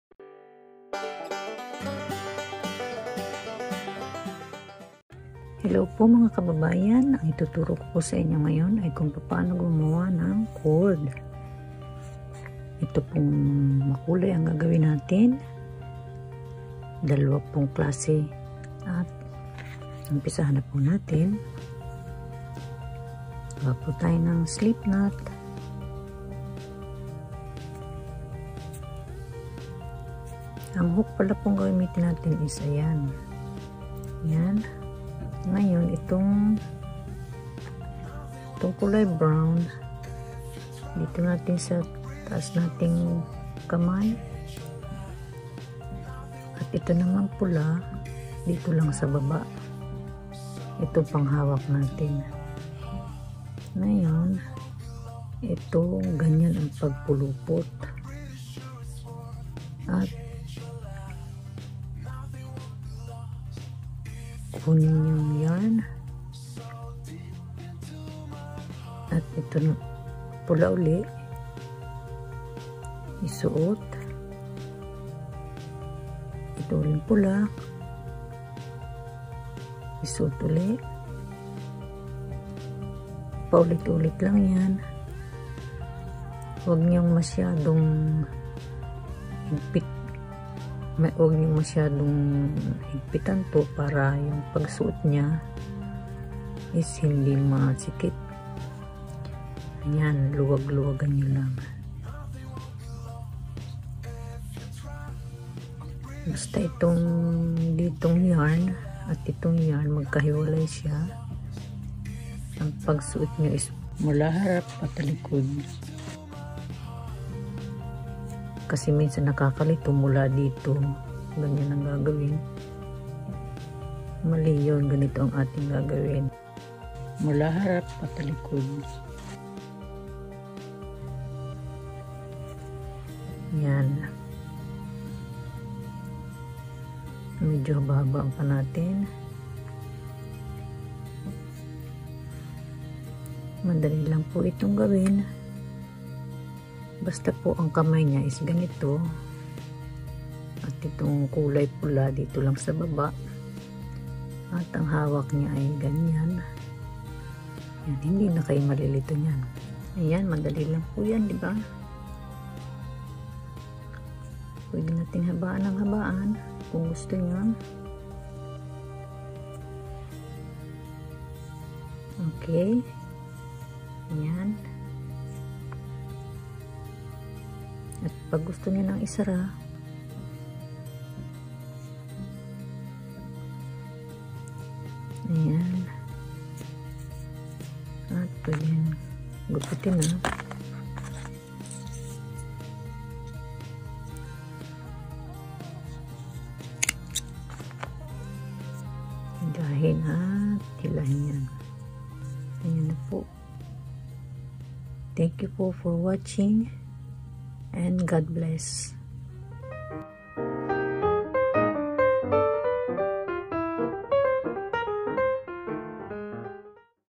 [gantsilyo] Paano gumawa ng Cord ng Bag
Easy crochet for beginners.

GOD BLESS!